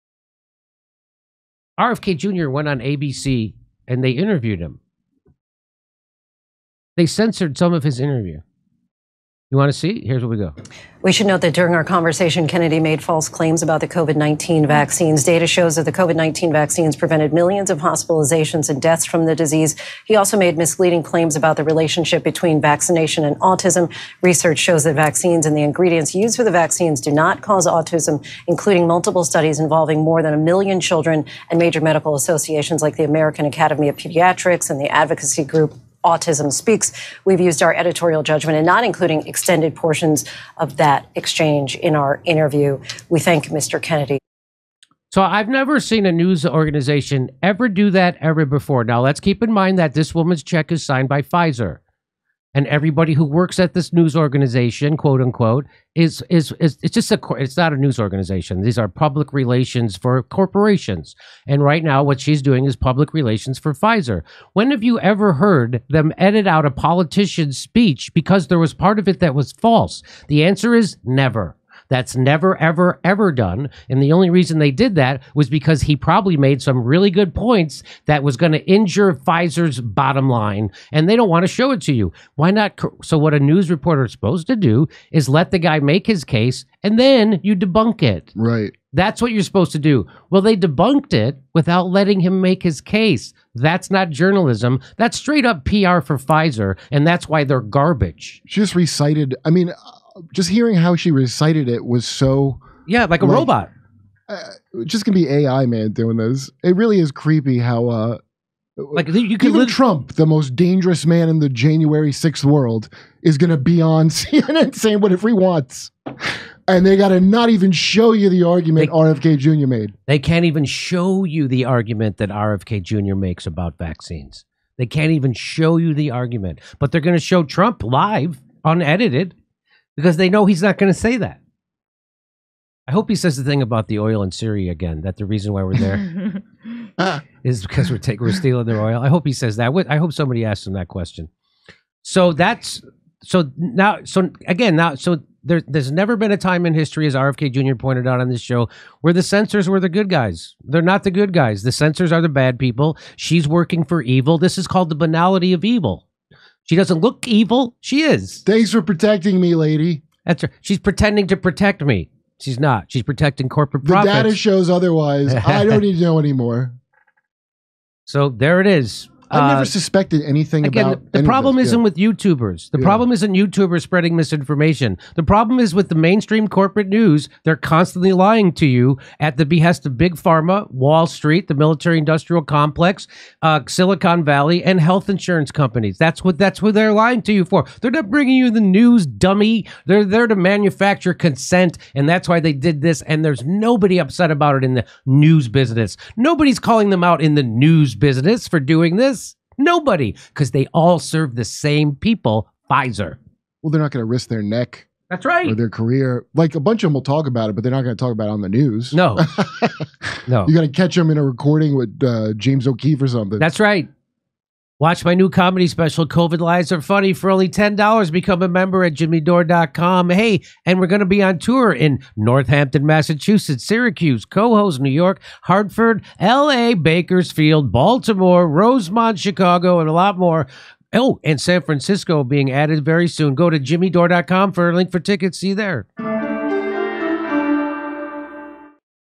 ABC Censors Its OWN Interview With RFK Jr.
After ABC News landed an interview with newly-announced presidential candidate Robert F. Kennedy Jr., the network did something decidedly odd: they chose not to air a portion of the interview and then announced afterwards that the conversation had been selectively censored because of some of the comments Kennedy had made about COVID, vaccines and public policy.

Jimmy and Americans’ Comedian Kurt Metzger discuss how bizarre it is for a news network to announce it is censoring an interview subject in this manner.